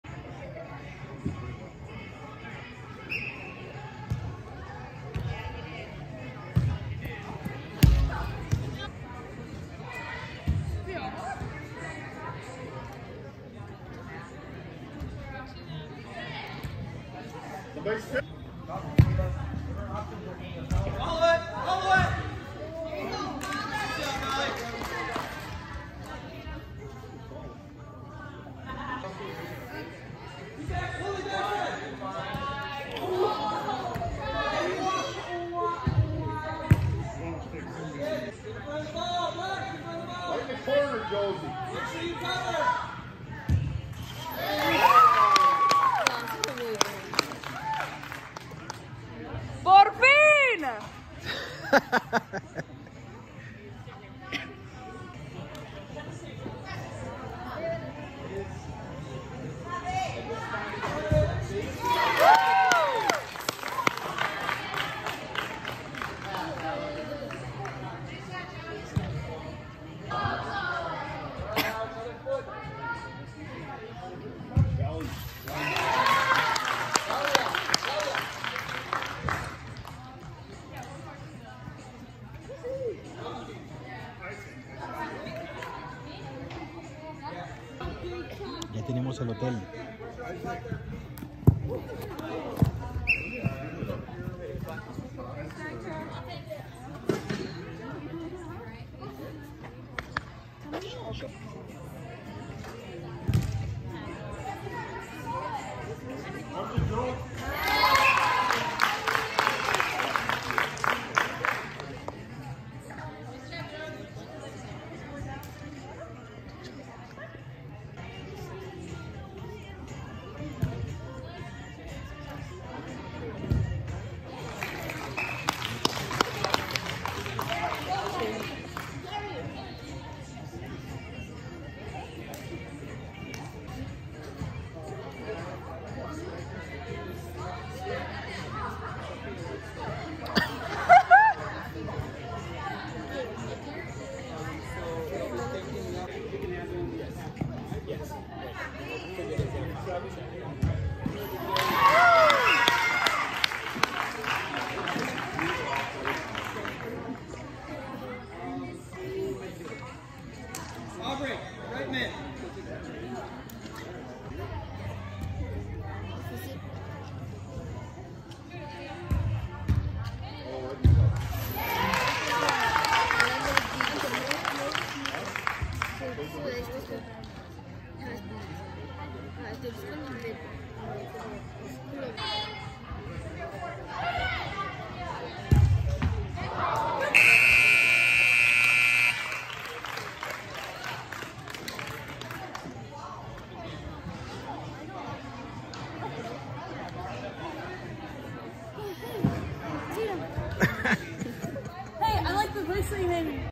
Highlights U13/14 Girls Indoor Soccer 2010 FC vs Loon-E Toones at Sports Emporium in DePere WI
Highlights U13_14 Girls Indoor Soccer 2010 FC vs Loon-E Toones at Sports Emporium in DePere WI 11-02-23